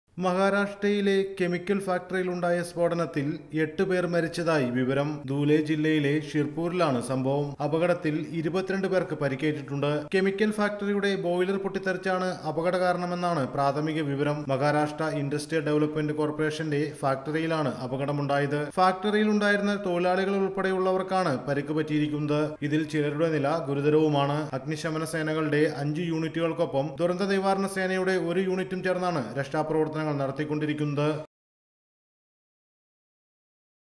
ഫാക്ടറിയിലെ ബോയിലർ പൊട്ടിത്തെറിച്ചു
മുംബൈ: മഹാരാഷ്ട്രയിലെ ധൂലെയിൽ കെമിക്കൽ ഫാക്ടറിയിൽ വൻ സ്‌ഫോടനത്തിൽ 8 മരണം. 22 പേർക്ക് പരിക്കേറ്റു. ഫാക്ടറിയിലെ ബോയിലർ പൊട്ടിത്തെറിച്ചാണ് അപകട കാരണം.